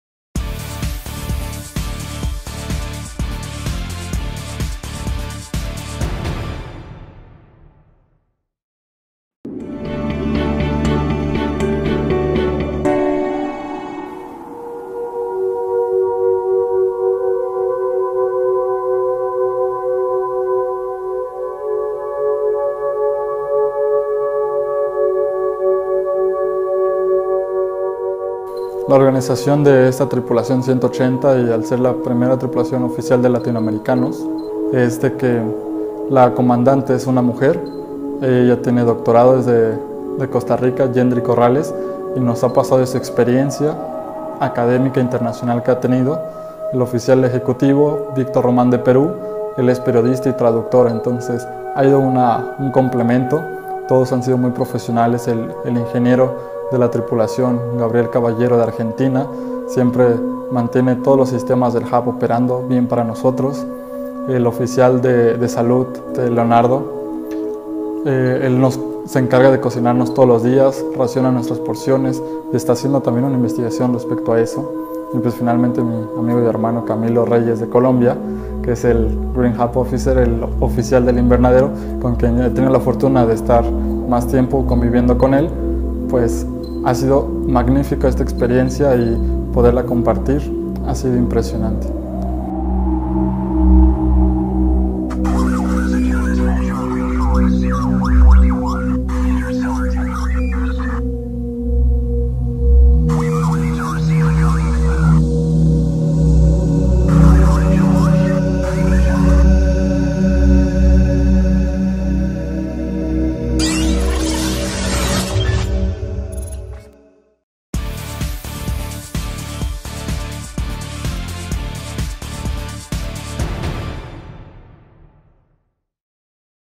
La misión LATAM I propone crear Agencia Espacial Latinoamericana
Caminar por el desierto para conquistar el espacio, soportar climas extremos, estar lejos de sus patrias y familiares. Los seis latinoamericanos de la tripulación comparten el idioma, la pasión por el conocimiento, el sueño de conquistar Marte y las mismas preocupaciones sobre los problemas sociales que atraviesan sus respectivos países.